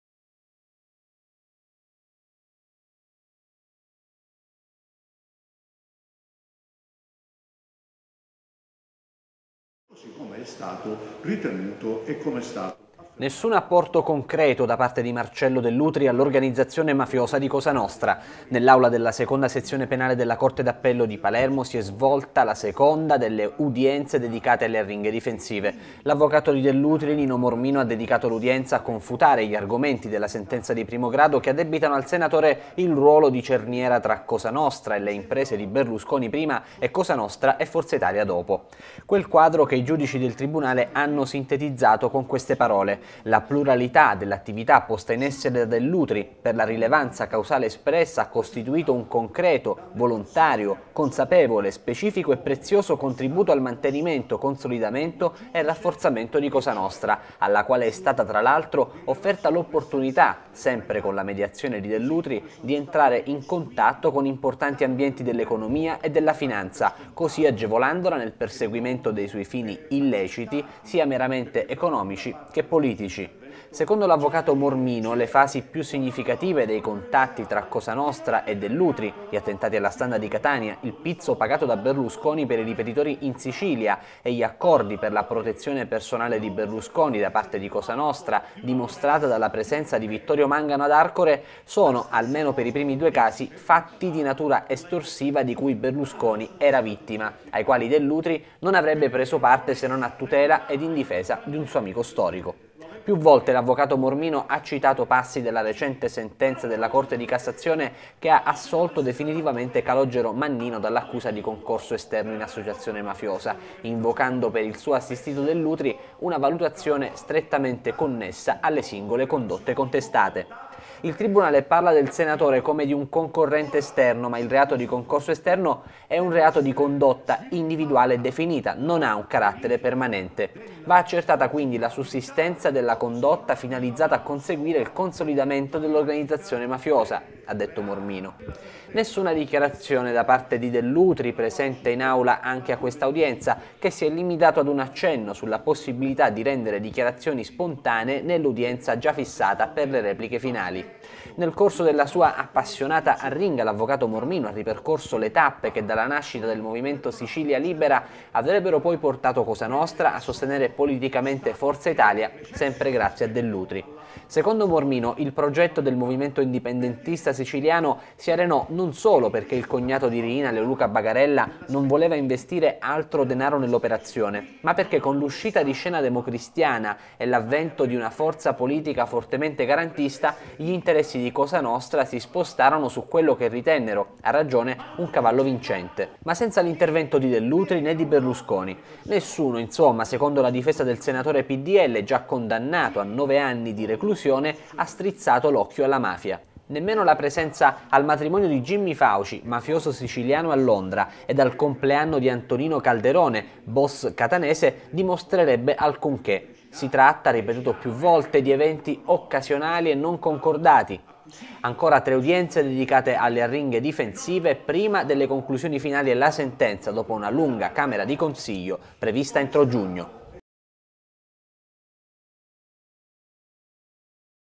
Processo Dell'Utri: "coi mafiosi solo rapporti occasionali"
"Nessun apporto concreto da parte di Marcello Dell'Utri all'organizzazione mafiosa di Cosa Nostra". Nell'aula della seconda sezione penale della Corte d'Appello di Palermo si è svolta la seconda delle udienze dedicate alla arringhe difensive.
L'avvocato di Dell'Utri, Nino Mormino, ha dedicato l'udienza a confutare gli argomenti della sentenza di primo grado che addebitano al senatore il ruolo di cerniera tra Cosa Nostra e le imprese di Berlusconi prima e Cosa Nostra e Forza Italia dopo.

Quel quadro che i Giudici del Tribunale hanno sintetizzato in queste parole.

La pluralità dell'attività posta in essere da Dell'Utri, per la rilevanza causale espressa, ha costituito un concreto, volontario, consapevole, specifico e prezioso contributo al mantenimento, consolidamento e rafforzamento di Cosa nostra, alla quale è stata, tra l'altro offerta l'opportunità, sempre con la mediazione di Dell'Utri, di entrare in contatto con importanti ambienti dell'economia e della finanza, così agevolandola nel perseguimento dei suoi fini illeciti, sia meramente economici che politici.

Secondo l'avvocato Mormino le fasi più significative dei contatti tra Cosa Nostra e Dell'Utri, gli attentati alla Standa di Catania, il pizzo pagato da Berlusconi per i ripetitori in Sicilia, e gli accordi per la protezione personale di Berlusconi da parte di Cosa Nostra dimostrata dalla presenza di Vittorio Mangano ad Arcore  sono - almeno per i primi due casi - fatti di natura estorsiva di cui Berlusconi era vittima ai quali Dell'Utri non avrebbe preso parte se non a tutela ed in difesa di un suo amico storico.

Più volte l'avvocato Mormino ha citato passi della recente sentenza della Corte di Cassazione che ha assolto definitivamente Calogero Mannino dall'accusa di concorso esterno, invocando per il suo assistito Dell'Utri, una valutazione strettamente connessa alle singole condotte contestate

 «Il tribunale parla del senatore come di un 'concorrente esternò, ma il reato di concorso esterno è un reato di condotta individuale e definita, non ha un carattere permanente. Va accertata la sussistenza delle condotta finalizzata a conseguire il consolidamento dell'organizzazione mafiosa».

Nessuna dichiarazione da parte di Dell'Utri, presente in aula anche a questa udienza, che si è limitato ad un accenno sulla possibilità di rendere dichiarazioni spontanee nell'udienza già fissata per le repliche finali.

Nel corso della sua appassionata arringa l'avvocato Mormino ha ripercorso le tappe che, dalla nascita del movimento Sicilia Libera, avrebbero poi portato Cosa Nostra a sostenere politicamente Forza Italia, sempre grazie a Dell'Utri.

Secondo Mormino il progetto del movimento indipendentista siciliano si arenò non solo perché il cognato di Riina, Leoluca Bagarella, non voleva investire altro denaro nell'operazione, ma perché con l'uscita di scena democristiana e l'avvento di una forza politica fortemente garantista, gli interessi di Cosa Nostra si spostarono su quello che ritennero, a ragione, un cavallo vincente.

Ma senza l'intervento di Dell'Utri né di Berlusconi. Nessuno, insomma, secondo la difesa del senatore pdl, già condannato a nove anni di reclusione, ha strizzato l'occhio alla mafia.

Nemmeno la presenza al matrimonio di Jimmy Fauci, mafioso siciliano a Londra, ed al compleanno di Antonino Calderone, boss catanese, dimostrebbero alcunché. Si tratta - ha ripetuto più volte - di eventi occasionali non concordati.

Ancora tre udienze dedicate alle arringhe difensive prima delle conclusioni finali e la sentenza, dopo una lunga camera di consiglio, prevista entro giugno.